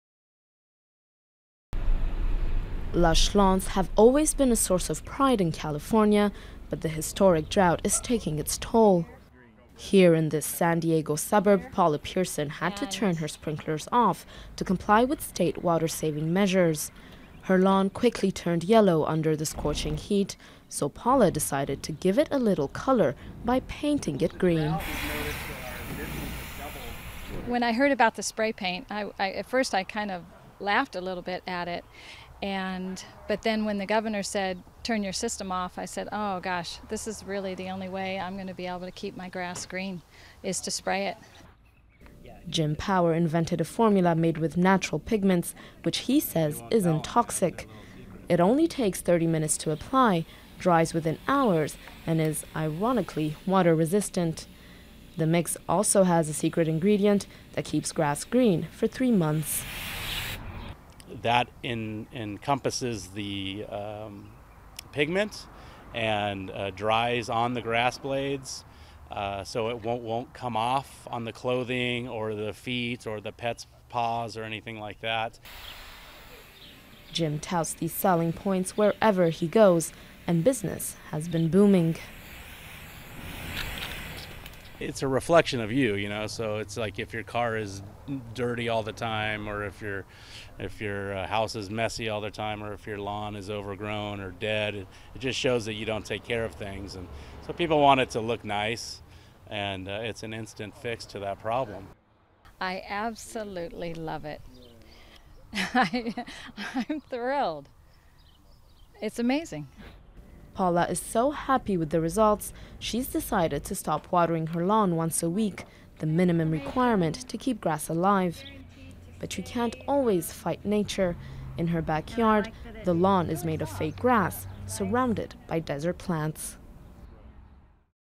Californians cope with drought by painting their lawns green
Facing a historic drought and tighter water restrictions, some homeowners in California spray-paint their lawns green for that perfect just-watered look.

Source: AFP news agency